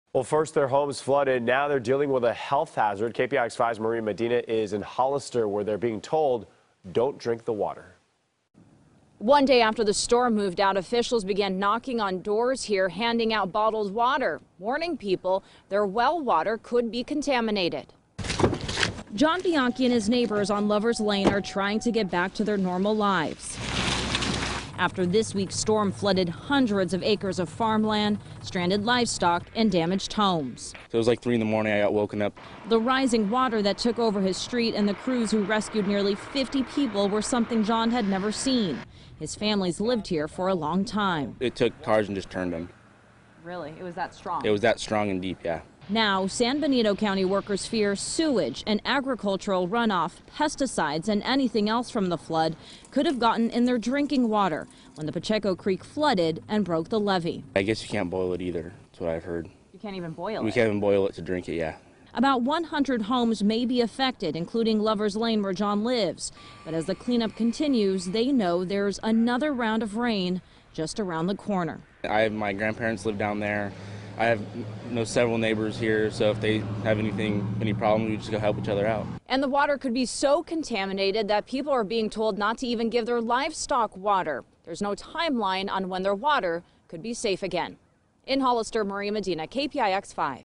Hollister Residents Warned Well Water Could Be Contaminated
Maria Medina reports on residents in Hollister being warned not to drink tap water due to possible contamination (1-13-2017)